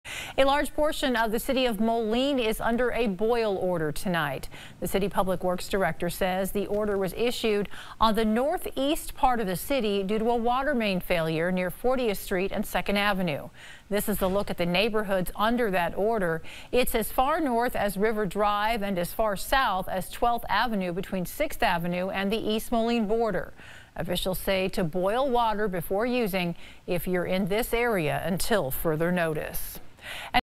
Major water main failure puts part of city under boil order, Moline city officials say
A boil order has been issued for the North East part of Moline, according to a media release from city officials.